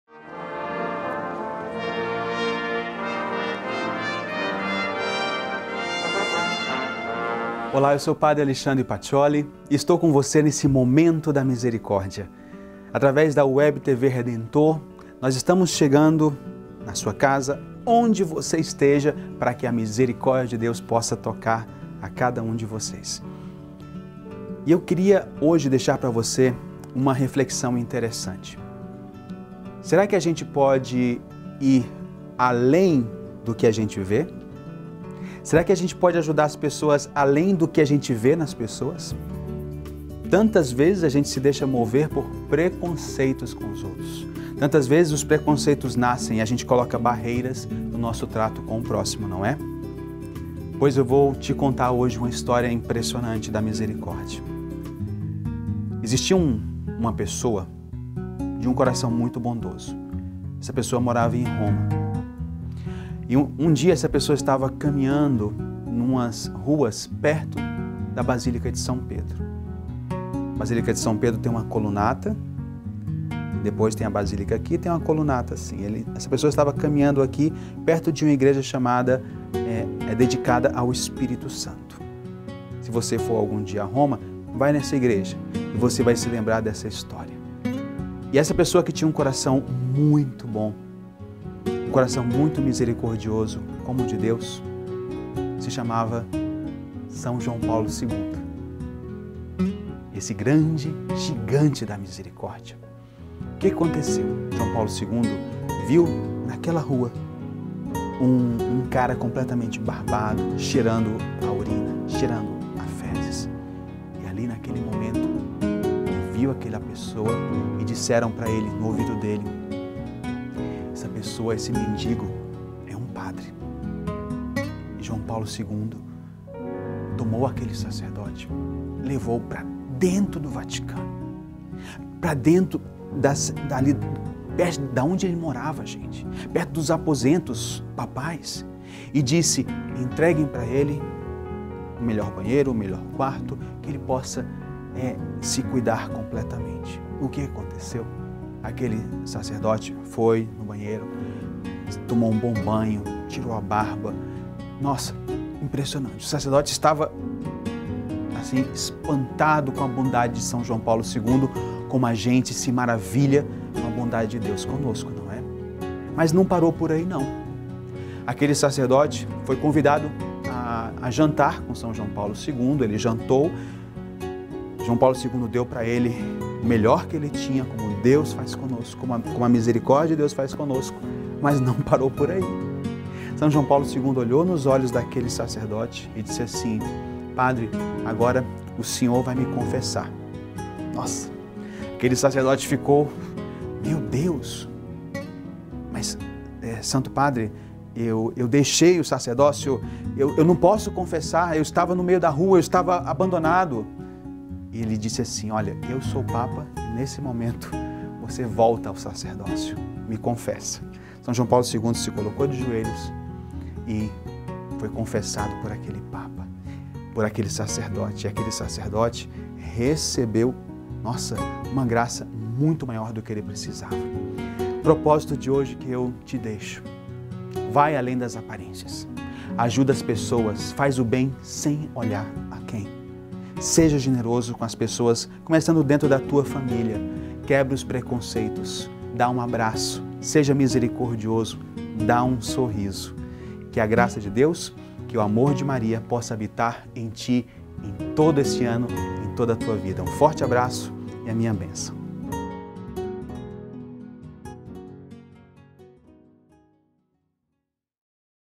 Momento da Misericórdia
Toda a terça-feira, o Padre Alexandre Paccioli vai contar um testemunho da Misericórdia.